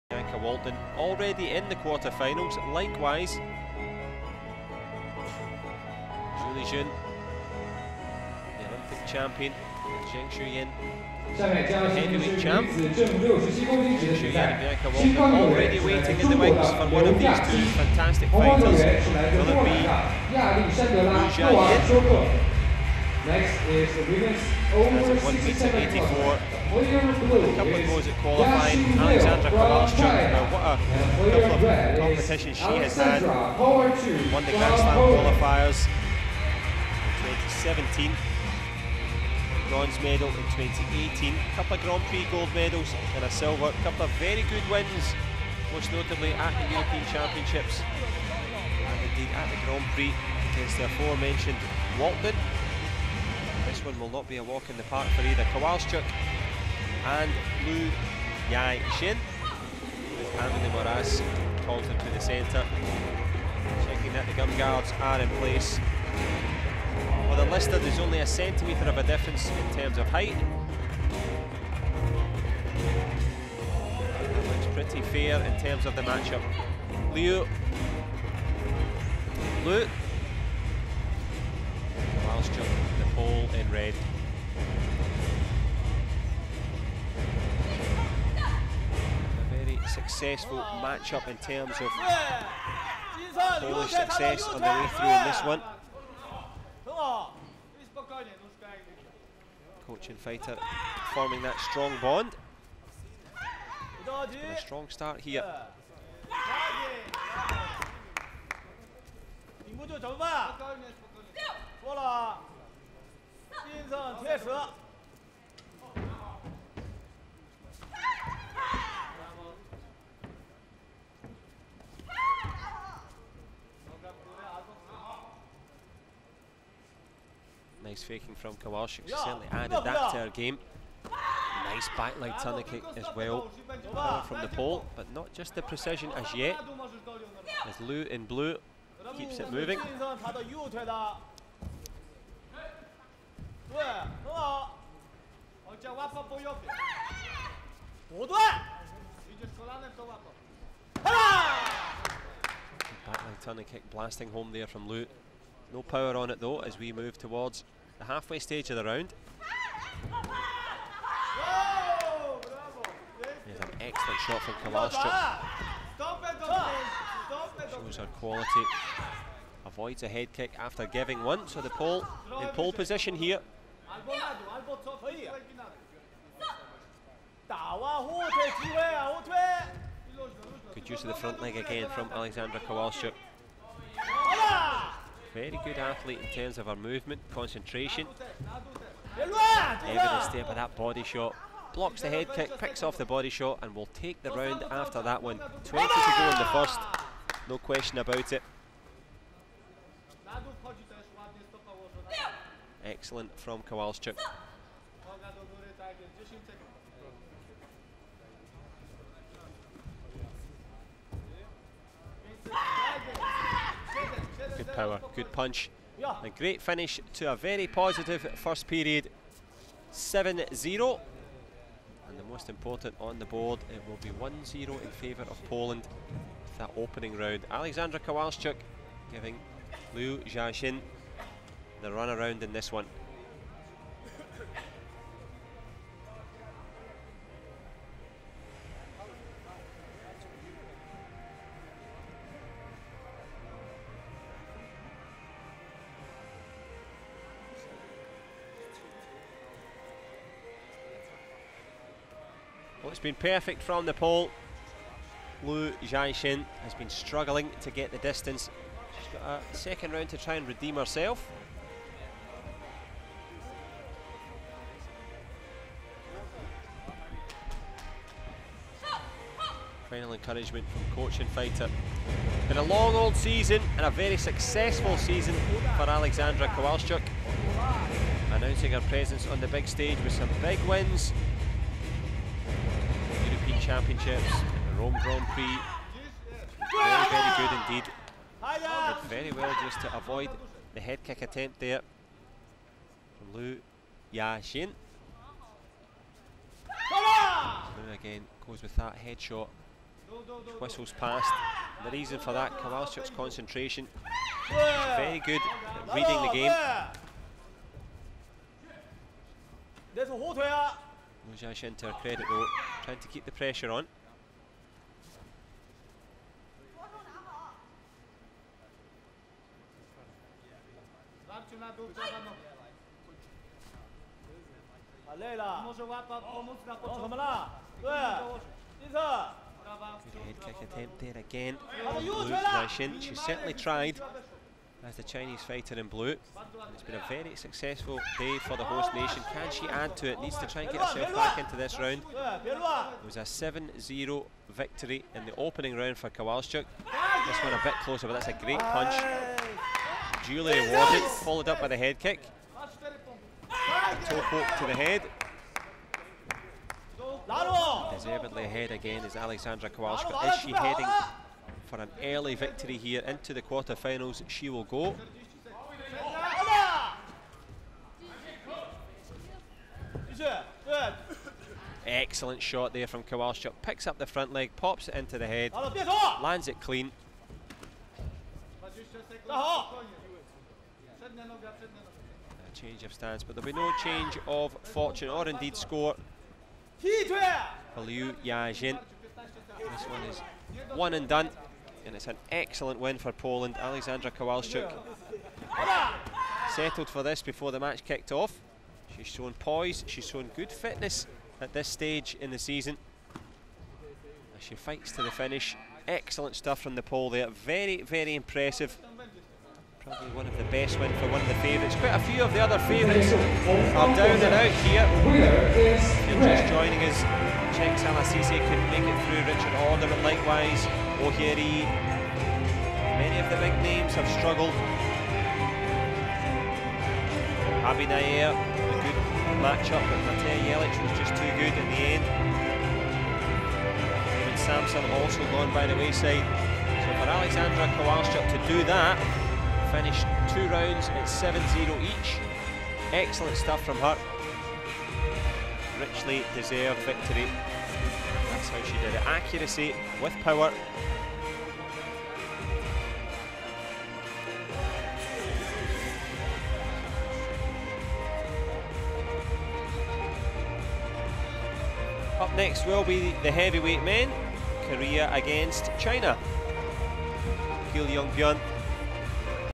F+67kg Round of 16, Yaxin LIU (CHN) VS Aleksandra Kowalczuk (POL)
Wuxi 2018 World Taekwondo Grand Slam Champions Series